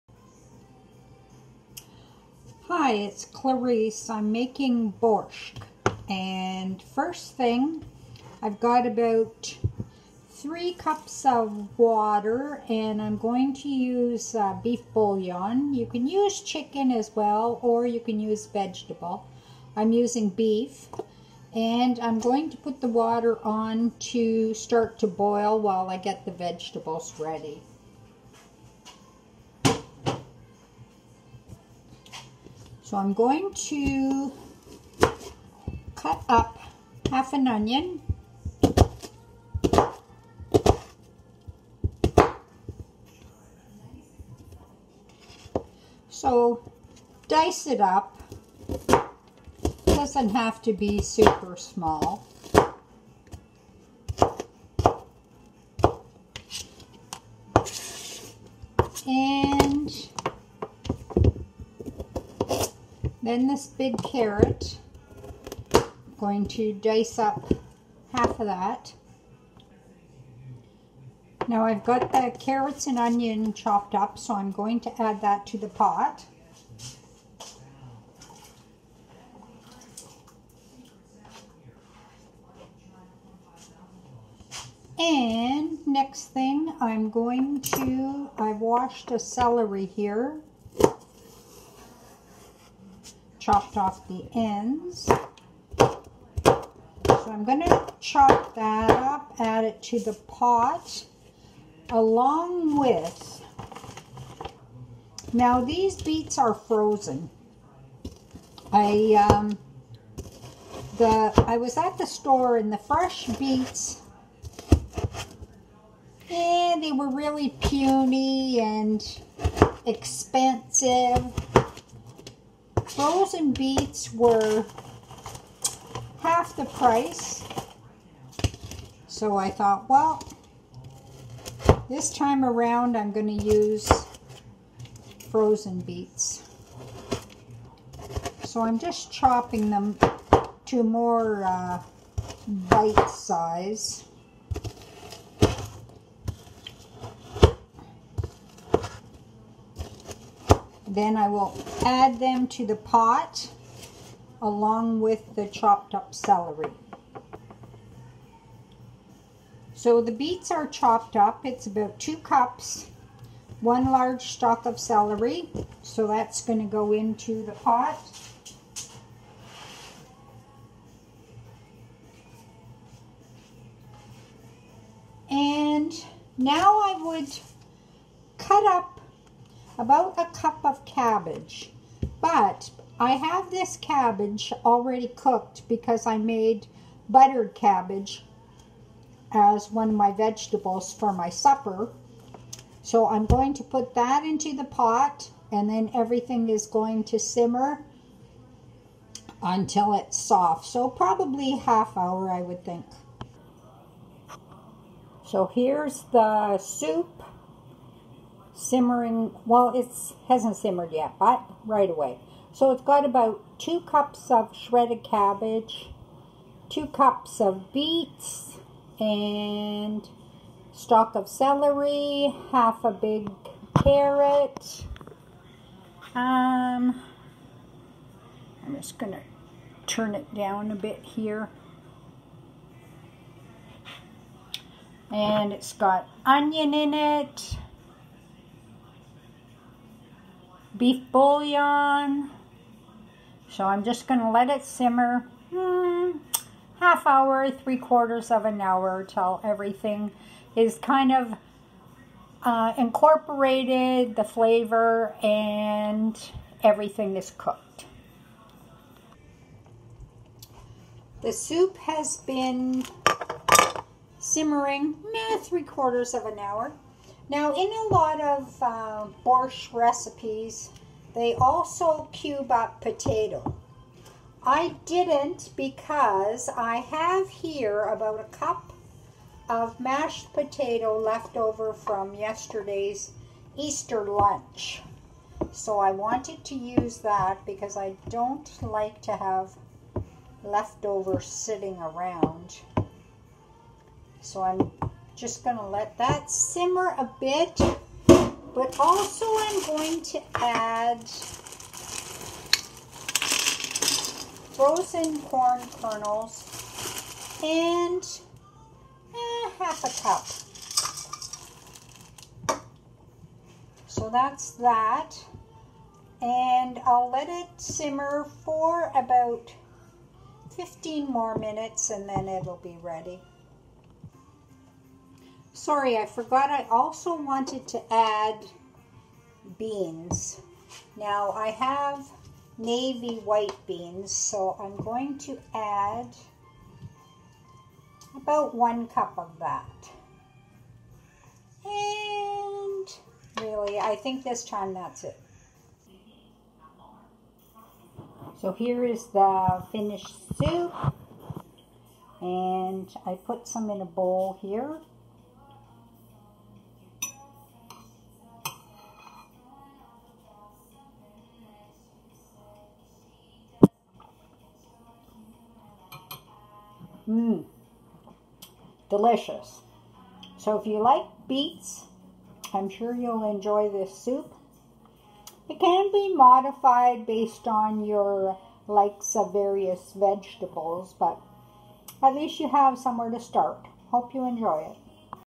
How to make Borscht
Ingredients
2 diced potatoes, 2 peeled and diced beets or 1 can, 1 small diced onion, 1 carrot peeled and diced, 1celery stalk diced, half cup corn kernels, 1 cup shredded cabbage,  3 bouillon cubes (beef, chicken or vegetable) 3 cups of water, 1 can white beans not drained

Directions: Put water and bouillon in stock pot. Add vegetables and bring to a boil, reduce heat and simmer until vegetables are cooked. Add beans during last 10 minutes, serve with a dollop of sour cream.
* You can turn this vegetarian dish into a meat dish by simmering stew meat with vegetables. Sauté beef in oil to brown before adding to pot or you can use soup bones or a spicy sausage such as kielbasa.